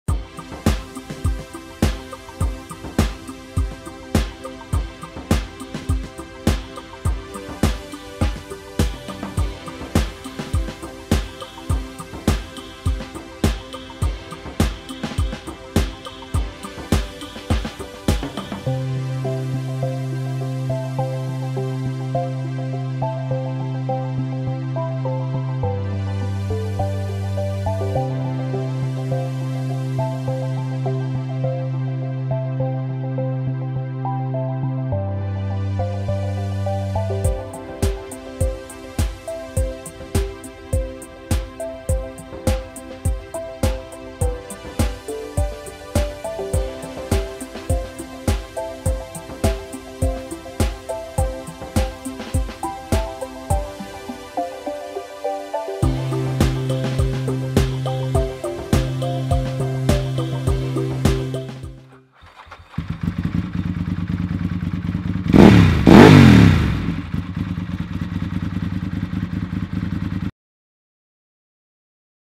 2000 Ducati 748R - 2023 Shannons Winter Timed Online Auction
The 2023 Shannons Winter Timed Online Auction running from 15 August to 22 August 2023, features a 2000 Ducati 748R.

- Lovely original condition and 40,970km on odometer
- Current custodian since 2012
- Factory toolkit